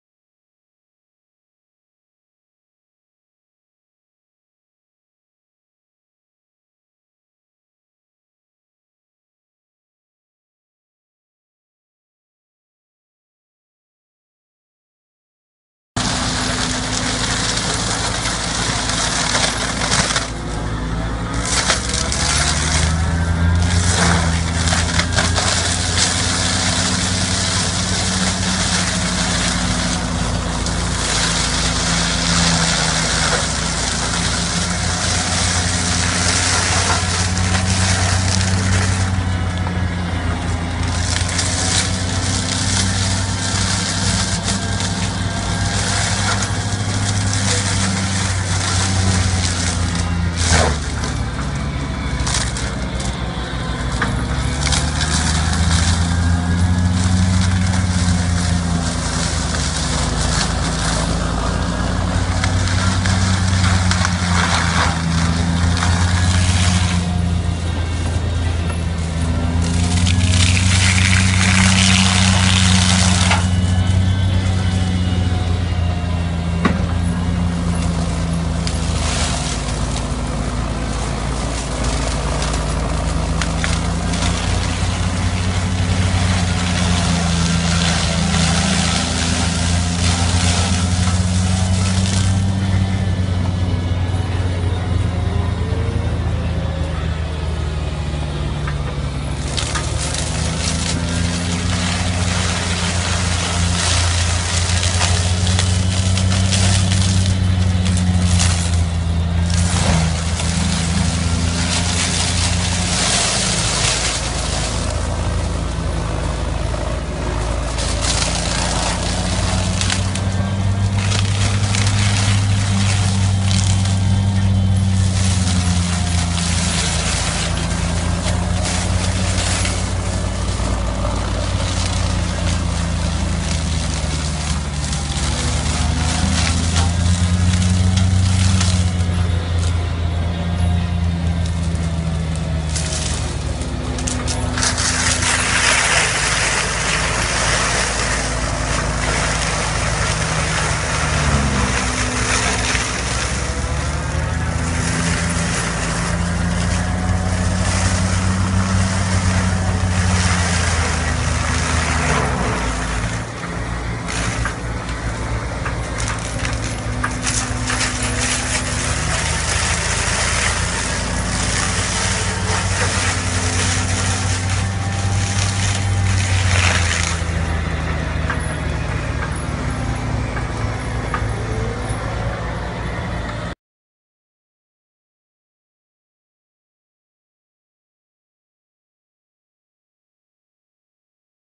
Trituradora Retro forestal - Trituradoras Picursa
La trituradora de brazo RETRO FORESTAL esta especialmente diseñada para la trituración de ramas de restos de podas, arboles bajos, restos que resultan de las talas de arboles, desbroces de montes y parcelas. Su diseño hace posible que se pueda colocar en cualquier modelo de retro y se coloca con motor hidráulico en función de los litros de caudal y la presión de cada retro.